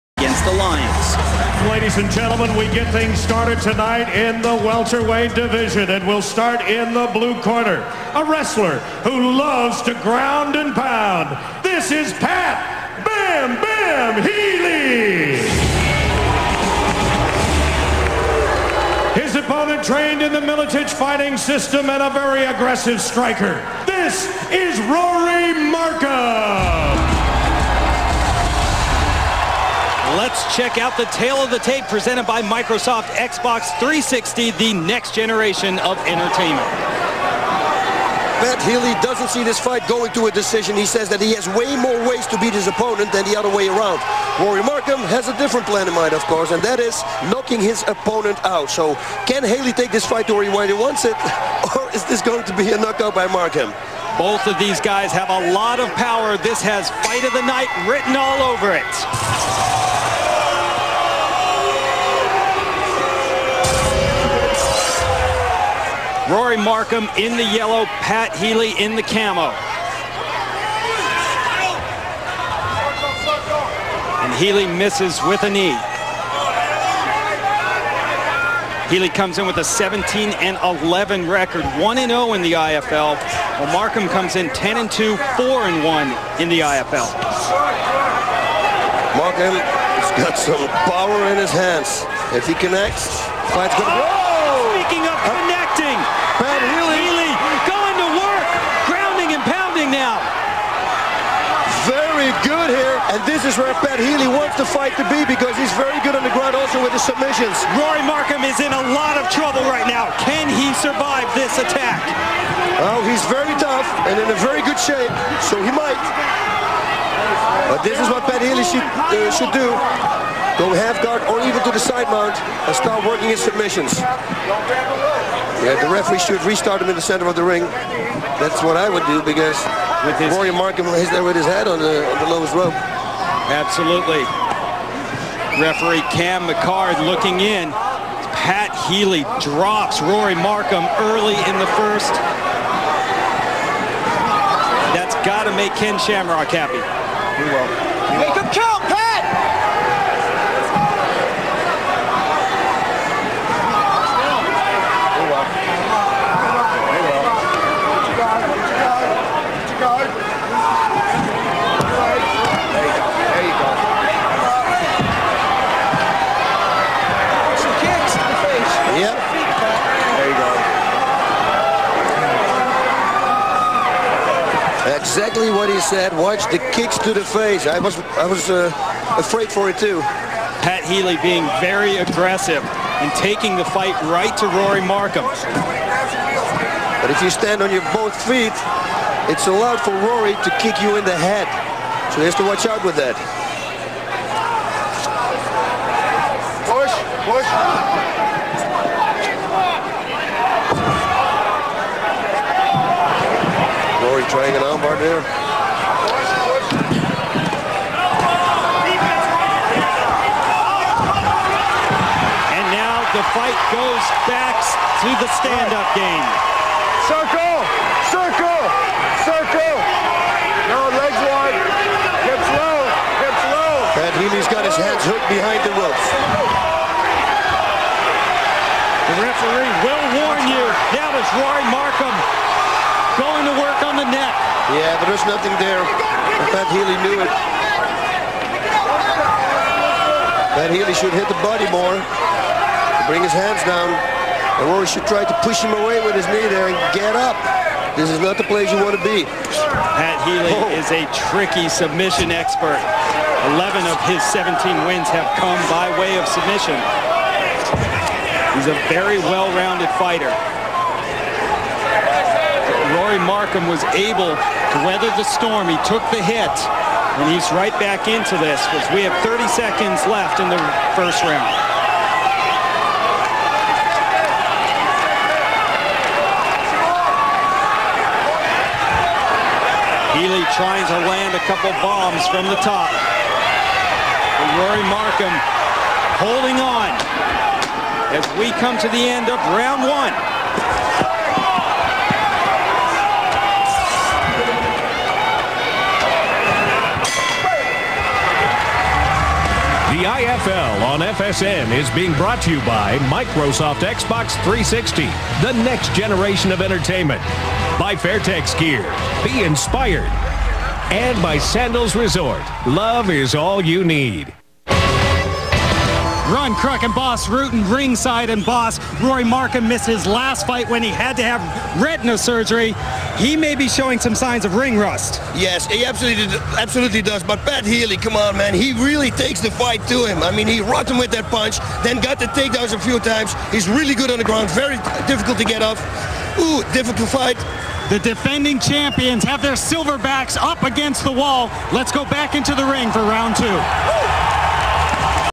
Rory Markham v.s Pat Healy Round 1, IFL
This is the first round of a three round war that took place in the International Fight League. What a fight!